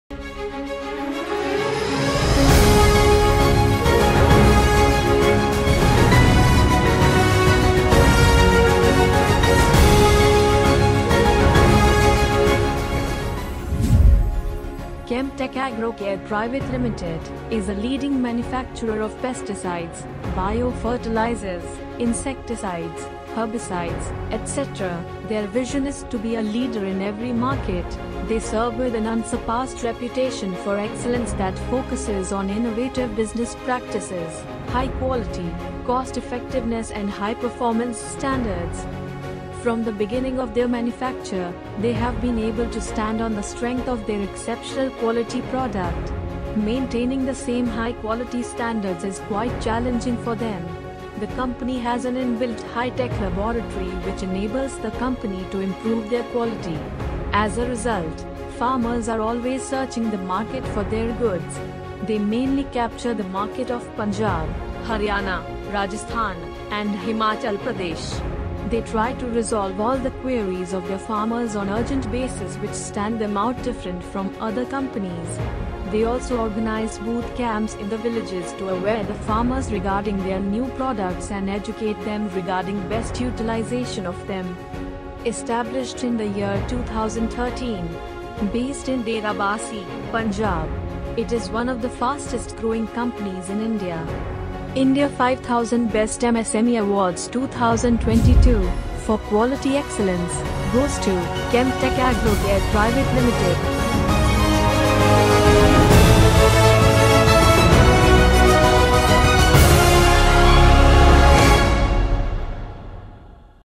Chemtech Agro Care Pvt. Ltd. - Winner of India 5000 Best MSME Awards 2022.
Chemtech Agro Care Pvt. Ltd. is a leading manufacturer of Pesticides, Bio Fertilizers, Insecticides, Herbicides, etc. Their vision is to be a leader in every market they serve with an unsurpassed reputation for excellence that focuses on innovative business practices, high quality, cost effectiveness and high-performance standards.

From the beginning of their manufacture, they have been able to stand on the strength of their exceptional quality product. Maintaining the same high-quality standards is quite challenging for them. The company has an in-built high-tech laboratory which enables the company to improve their quality. As a result, farmers are always searching the market for their goods. They mainly capture the market of Punjab, Haryana, Rajasthan and Himachal Pradesh. They try to resolve all the queries of their farmers on urgent basis which stand them out different from other companies. They also organize booth camps in the villages to aware the farmers regarding their new products and educate them regarding best utilization of them.

Established in the year 2013, based in Derabassi, Punjab. It is one of the fastest growing companies in India.

India 5000 Best MSME Awards 2022, For Quality Excellence, Goes to, Chemtech Agro Care Pvt. Ltd.